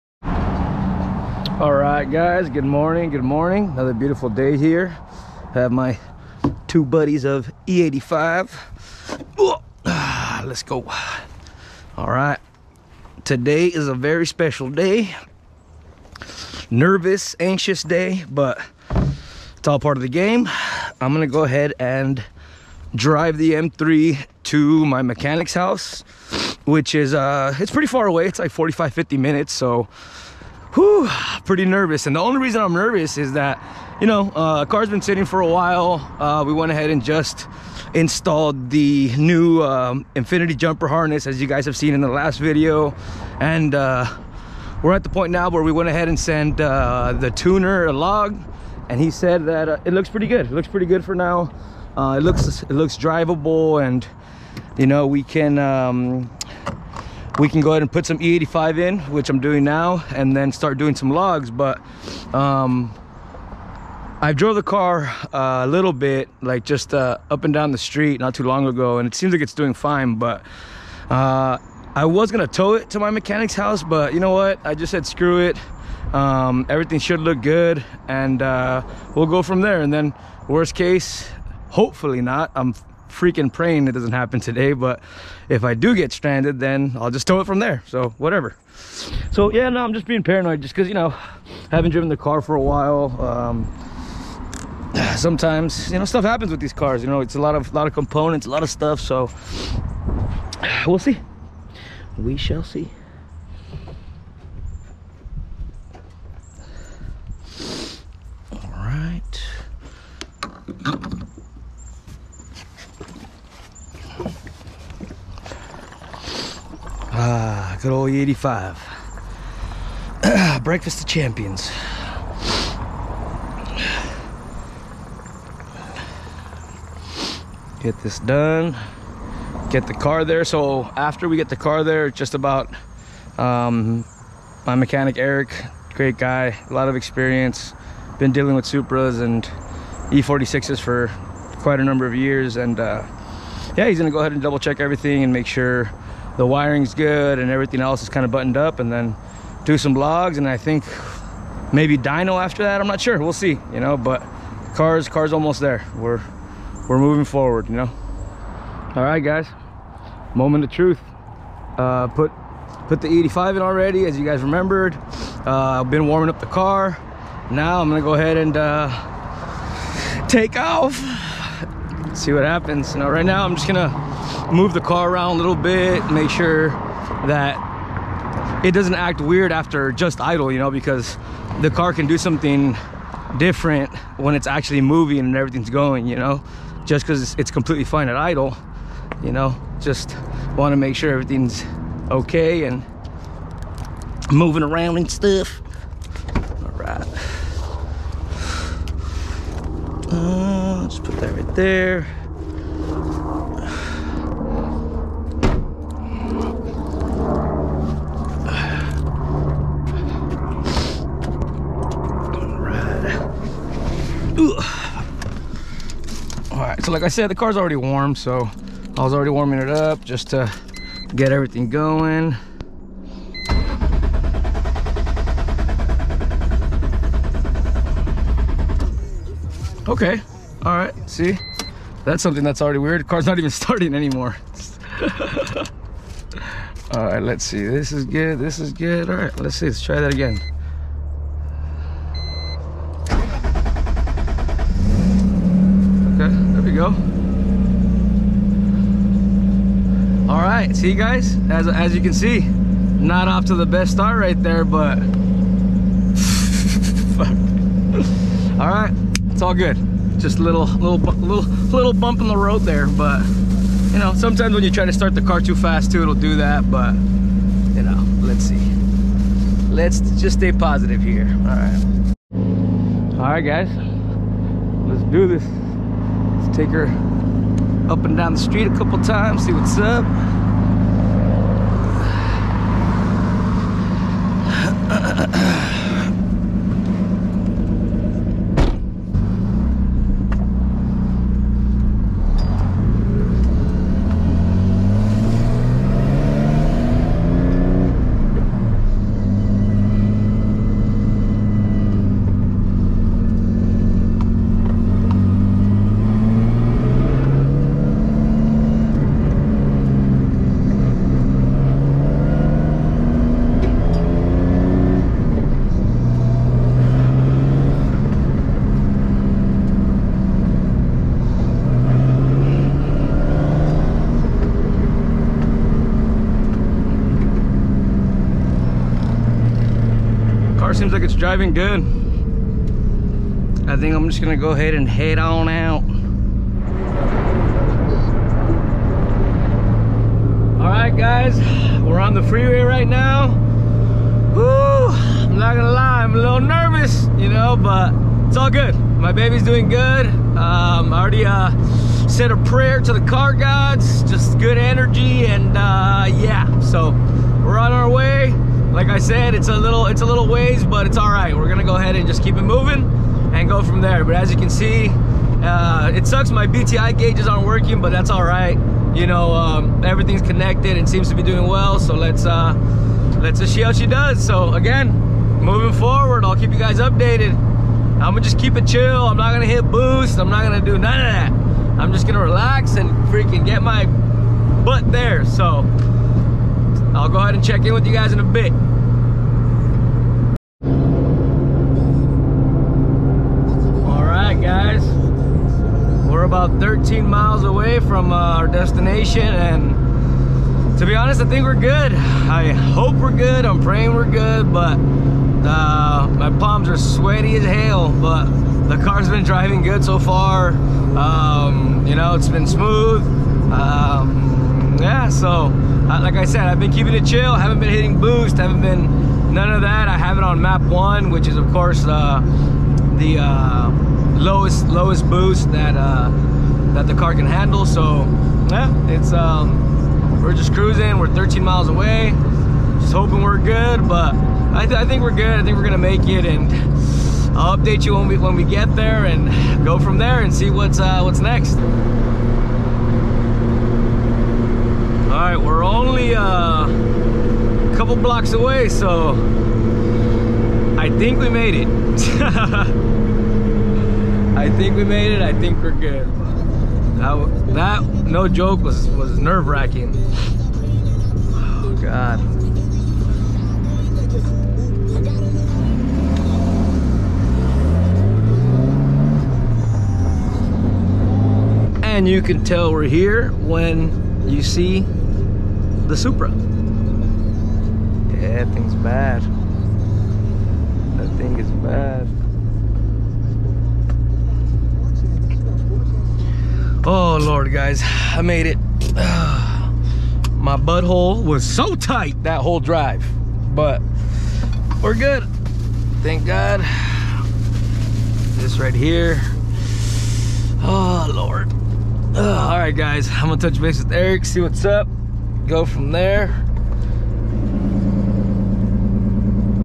Driving Turbo E46 M3 for 1st time in months!!
so in this video I drive my m3 for the 1st time in months! I was very nervous because the last few times I had driven the car I was getting stranded left and right due to random minor issues. check out the video and let me know what you guys think! Have some more content coming soon. also check out my IG @thewolfm3.